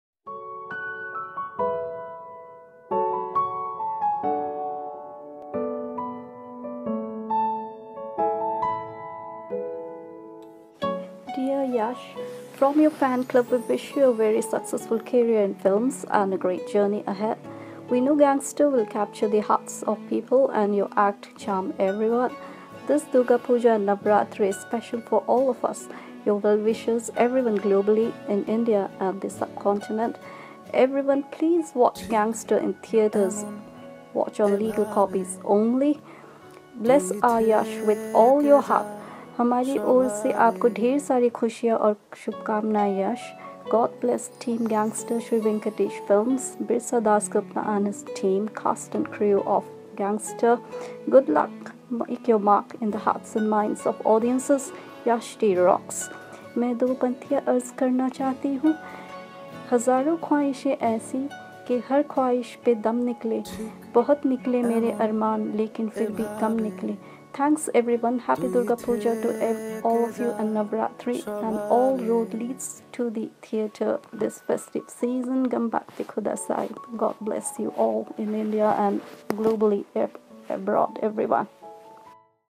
Fans Wish YashD on the eve of Gangster release- YashhD_FC
Yash Dasgupta kicks off his movie career with Gangster, directed by Birsa Dasgupta and produced by SriVenkatesh Films. On the eve of his debut movie release, Yash Dasgupta FanSpeaks invited his fans to send make videos with personal messages wishing Yash luck.